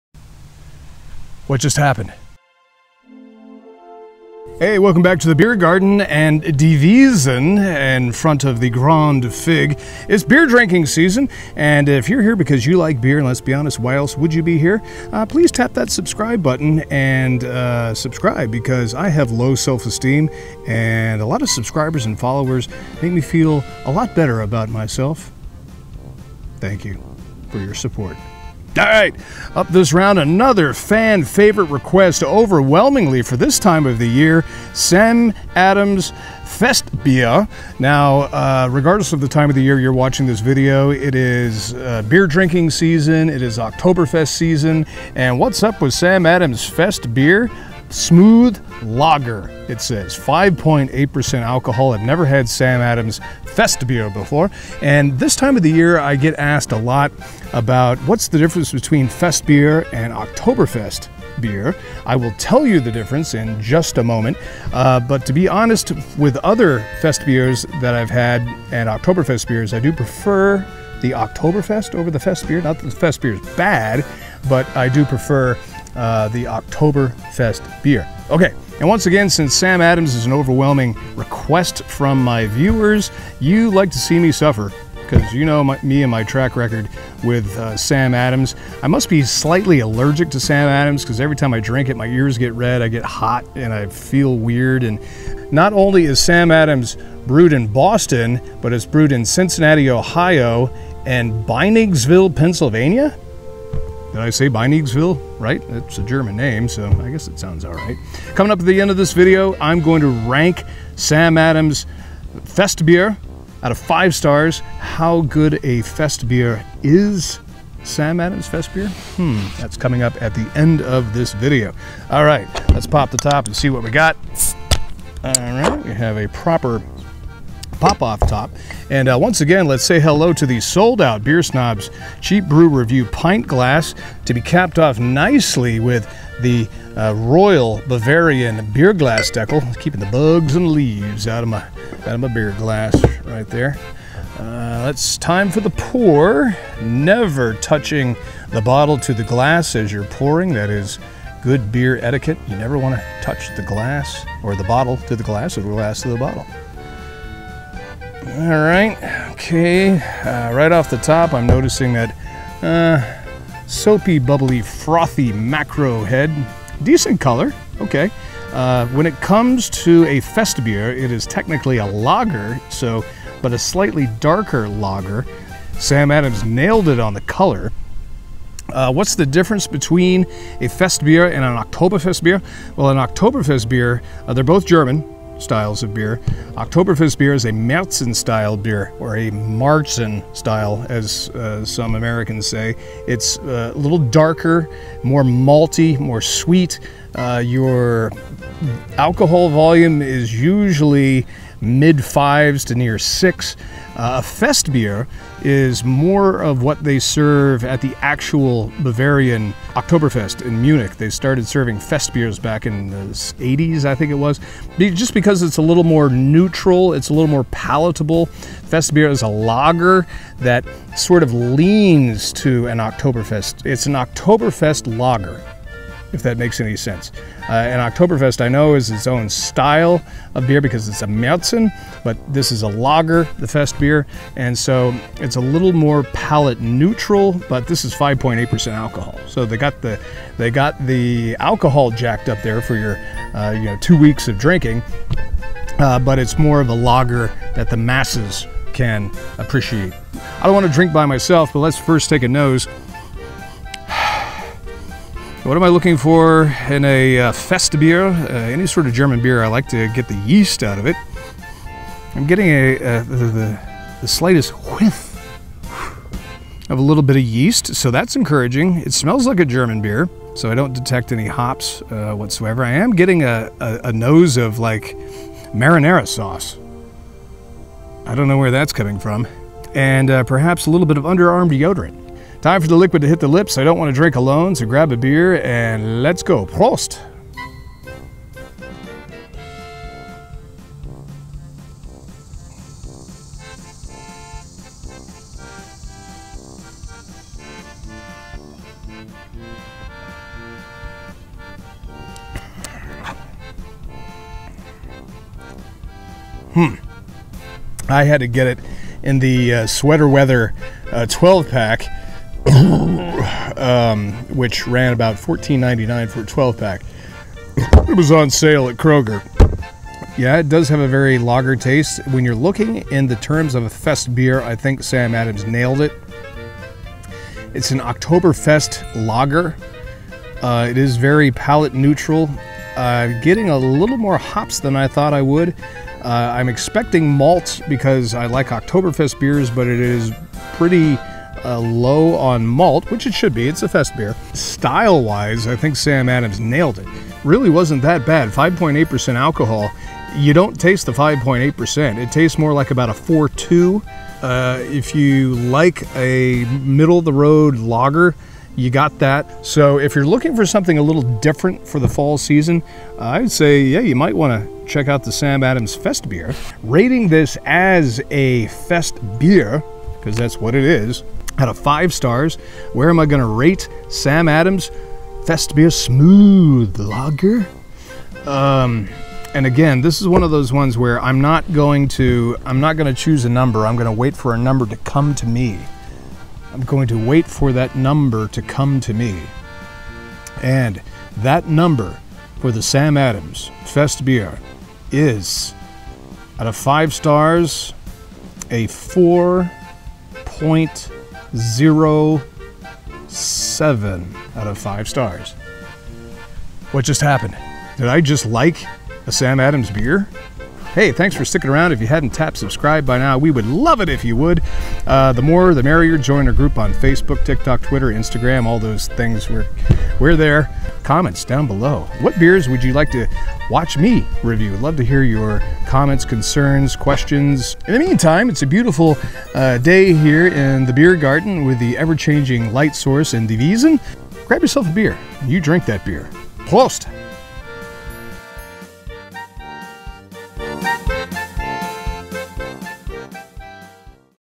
Sam Adams Festbier Beer Review 2022 (Fest Beer, Festbeer) by A Beer Snob's Cheap Brew Review
You won't believe what happens after I chug this 2022 version of Sam Adams Festbier! What's the difference between Festbier and Oktoberfestbier? I explain from Die Wiesn in The Beer Garden for Oktoberfest season! Prost!